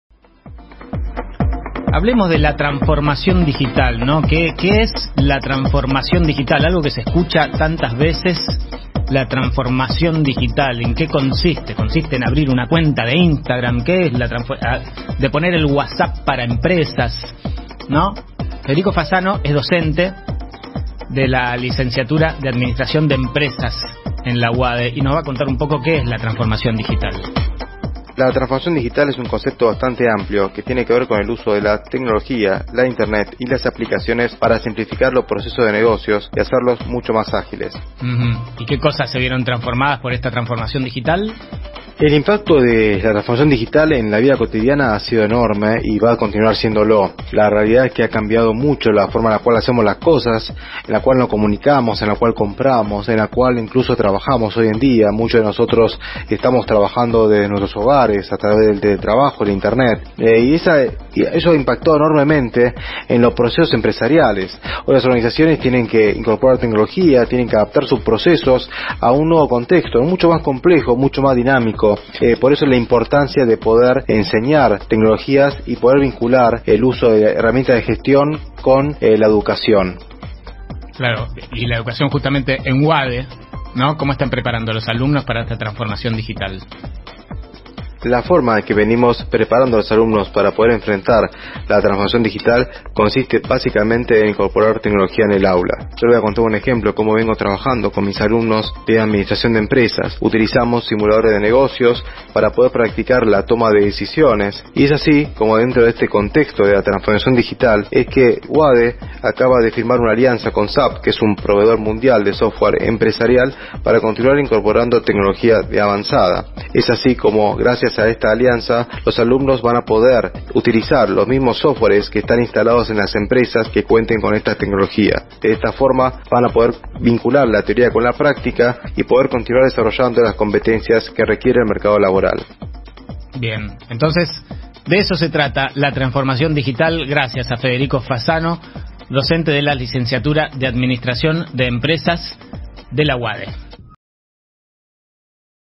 Administración de Empresas UADE: transformación digital
Federico Fasano, docente de la Lic. en Administración de Empresas de UADE, nos cuenta qué es la transformación digital en esta nota para Radio Berlín.

¿Te interesa este tema?